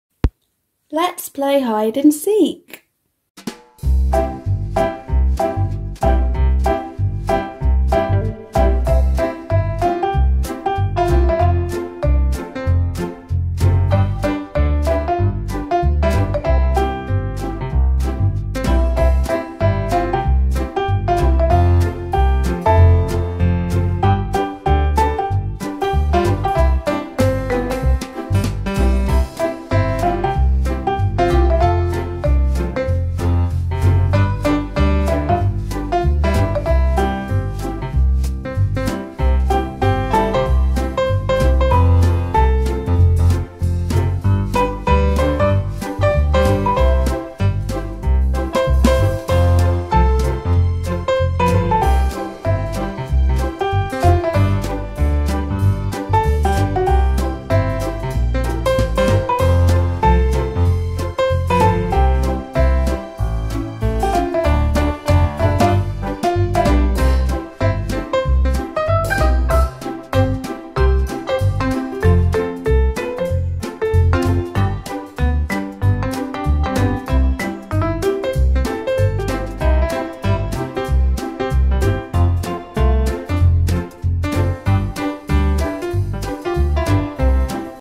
Find Freya at Brick Oven Bakery - Sylvanian Families/Calico Critters Hide & Seek Game
Find Freya at Brick Oven Bakery - Sylvanian Families/Calico Critters Hide & Seek Game | Amy's Animals: Secret Life Of Sylvanians

Sylvanian Families/Calico Critters Freya Chocolate, the Chocolate Rabbit Sister, is playing hide and seek at Brick Oven Bakery. Can you find Freya?

🌻🌻🌻 About Me 🌻🌻🌻